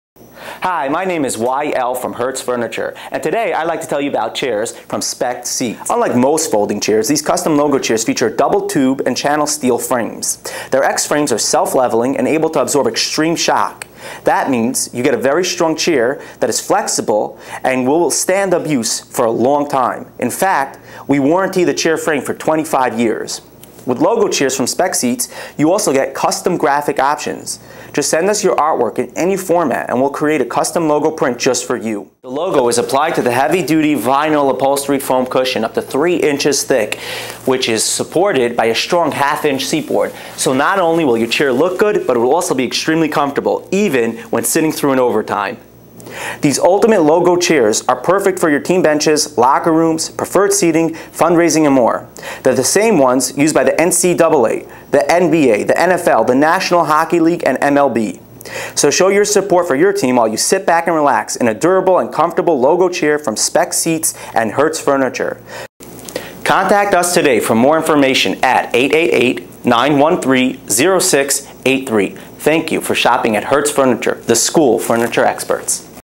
Spec Seats
are unlike other folding chairs.  They feature double tube and channel steel frames.